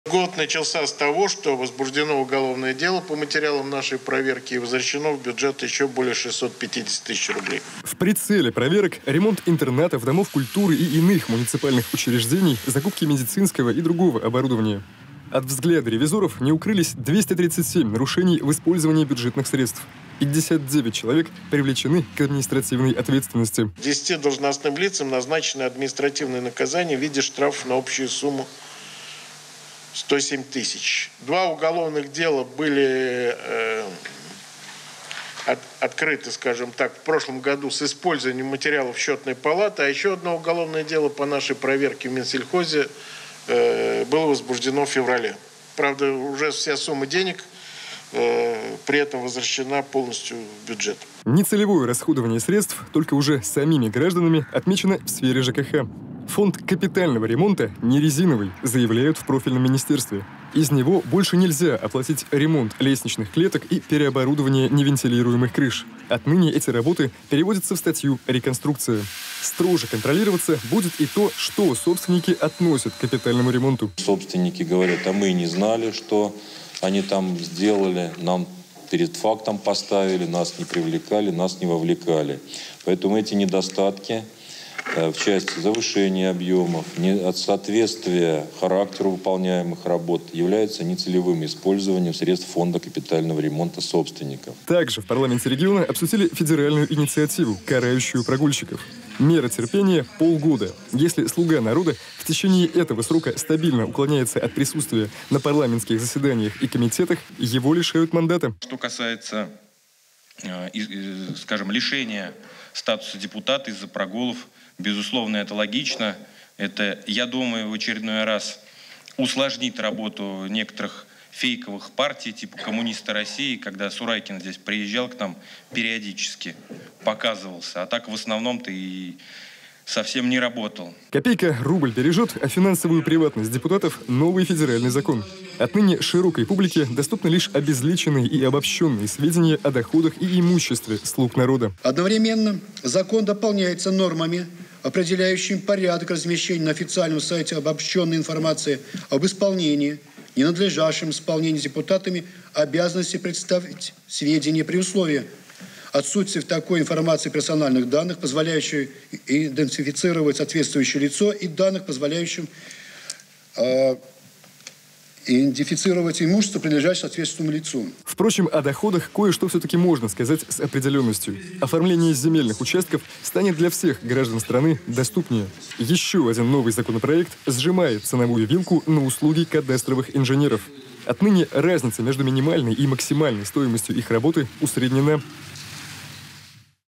Делу - время, деньгам - счёт. О новых инициативах регионального парламента
Счётная палата региона провела проверки, результатом которых стали несколько административных дел о нецелевом расходовании бюджетных средств в минувшем году. Но и без этого на заседании Законодательного собрания было, что послушать и о чём подумать. Модернизация требуется сфере ЖКХ, приведение в соответствие с федеральными нормами - областным законам.

О новых инициативах регионального парламента.⠀⠀⠀⠀⠀